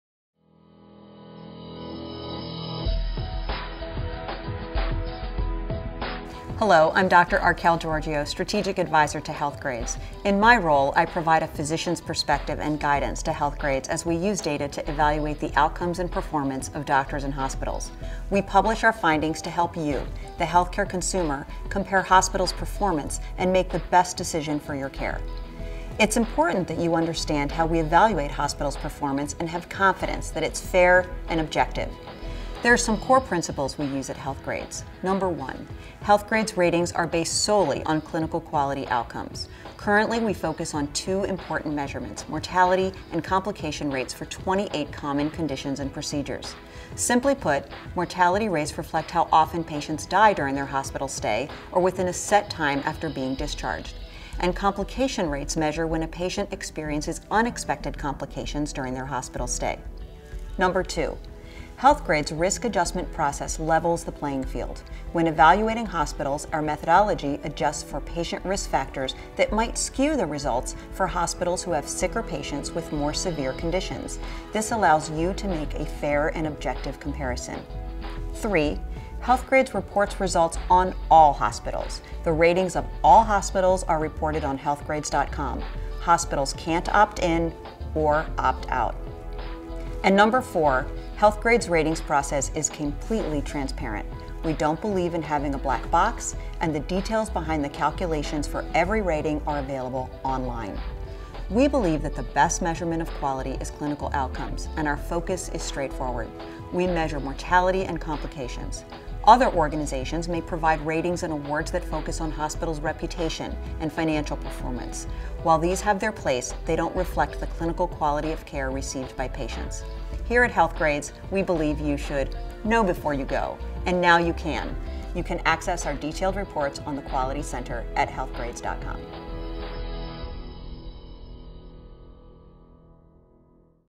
Why Quality Matters: Understanding Objective Measures of Performance | Healthgrades
Dr. Archelle Georgiou, MD, a strategic advisor to healthgrades.com, talks about the importance of objective measures of performance in healthcare ratings.

Healthgrades believes the best measure of quality is clinical outcomes and our focus is straightforward. We measure mortality and complications.

To learn more about the Healthgrades quality ratings, Be the first to see our new videos.